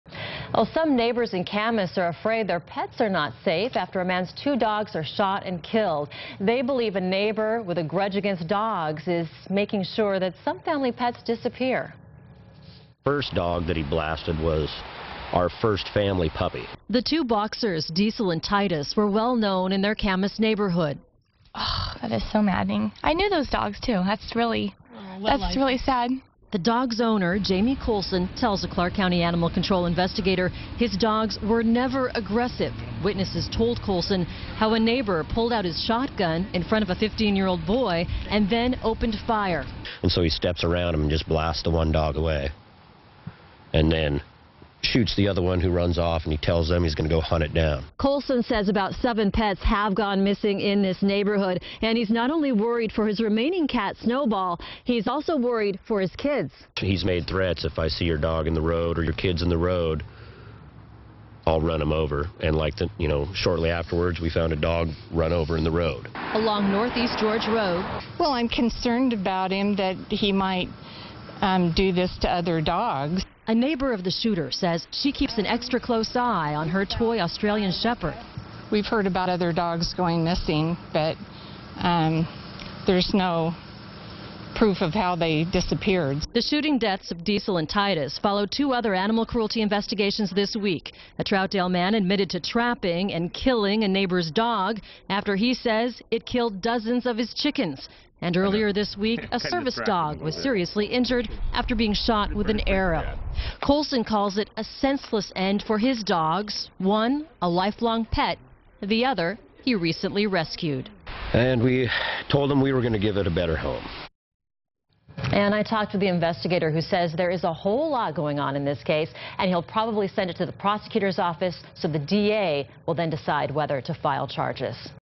Boxers shot, killed off NE George Road
CAMAS, Wash. -- Some neighbors in Camas are afraid their pets are not safe after a man's dogs are shot and killed. They believe a neighbor with a grudge against dogs is making sure some family pets disappear."The first dog he blasted was our first family puppy," said Jamey Colson. His two Boxers,